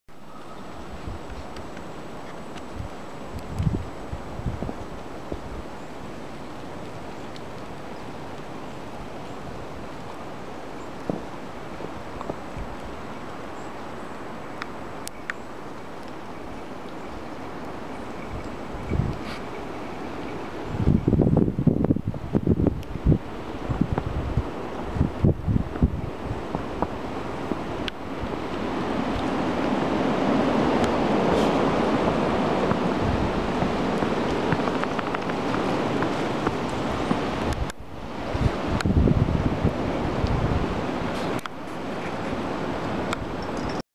風がぁぁっ！
吹き荒れた朝　雪も降ってきましたが　な～んだか暖かぃ♪